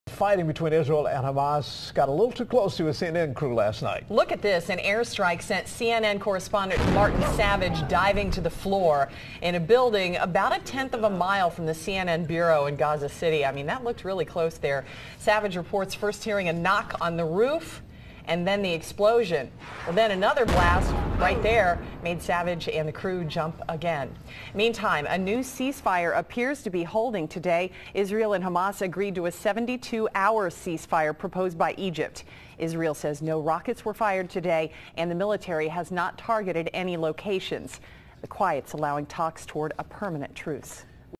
Airstrike is close call for CNN reporter
GAZA CITY (WKRC) -- Fighting between Israel and Hamas got a little too close to a CNN crew Sunday night.

An air strike sent CNN correspondent Martin Savidge diving to the floor in a building about a tenth of a mile from the CNN bureau in Gaza City.

Savidge reported first hearing a knock on the roof then the explosion.

A second blast later made Savidge and the crew jump again.

Israel and Hamas have agreed to a 72-hour cease-fire proposed by Egypt.

Israel says no rockets were fired Monday and the military hasn't targeted any locations.

The quiet's allowing talks toward a permanent truce.